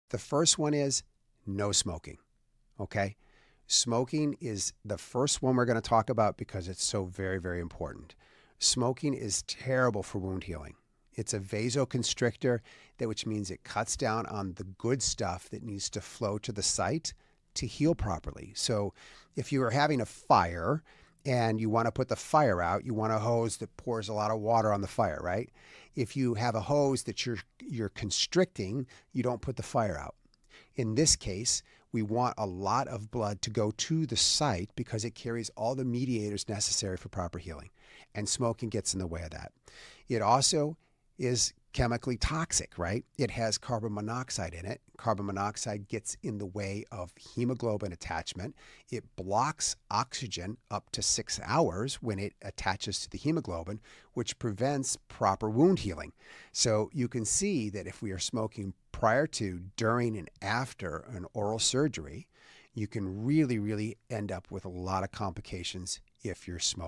Why Smoking Harms Wound Healing
After surgery, healing is everything. In this video, Dr. Robert Stanley, the Smile Engineer, breaks down the “4 S’s of Surgical Success” every patient needs to know:

No spitting
No straws
No smoking
No swishing

Each of these simple actions can create negative pressure or introduce irritation that disrupts blood clot formation and slows down recovery. Dr. Stanley explains why avoiding these behaviors is critical for proper healing and long-term surgical success. Whether you’re a patient recovering from dental surgery or a dentist educating your own patients, this video is a must-watch.